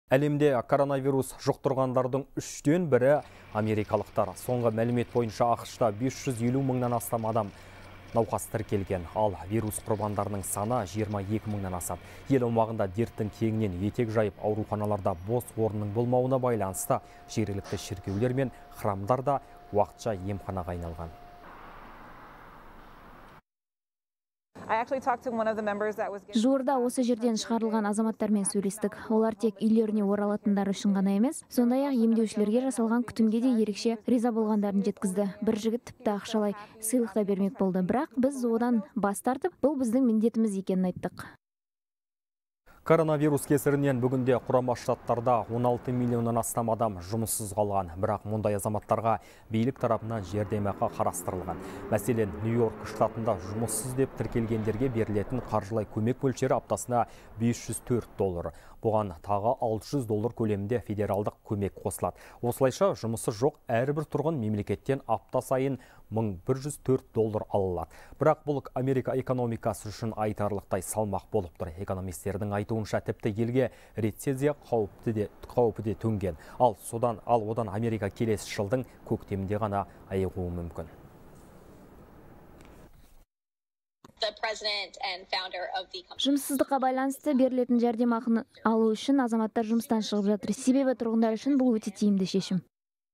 АҚШ-та жергілікті шіркеулер мен храмдар уақытша емханаға айналды
Әлемде коронавирус жұқтырғандардың үштен бірі – америкалықтар.